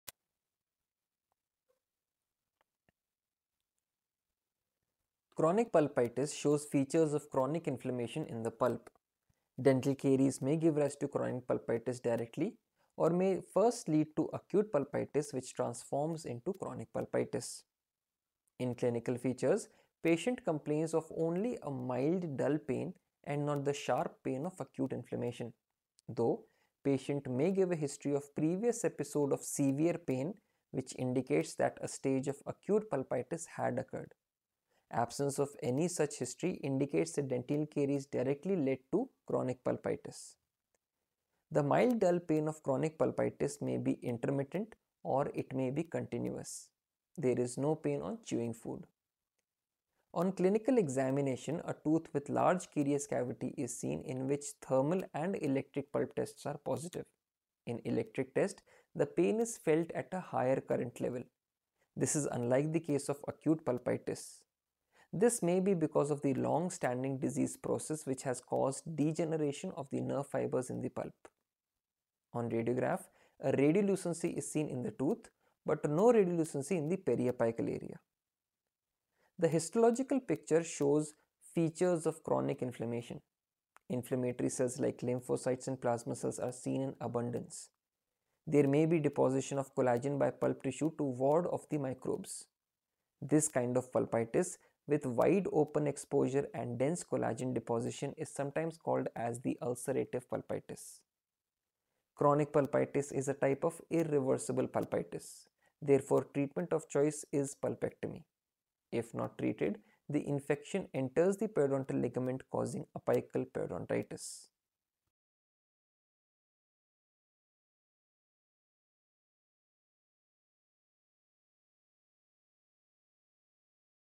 Chronic pulpitis - Diseases of pulp [Part 4]
This video on diseases of pulp highlights the condition known as Chronic pulpitis and its clinical, radiographic and microscopic features.

Our team:
Dr Shivendra Rana MDS
Dr Karishma Desai MDS
Dr Uma Datar Chougule MDS